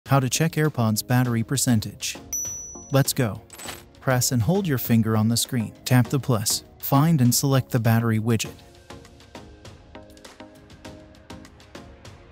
How to check Airpods battery percentage?
Simple and very step-by-step video how to check airpods battery percentage!

Here’s what you need to do to make this work:
Press and hold your finger on the screen,
tap the plus
in the top left corner
find and select the battery widget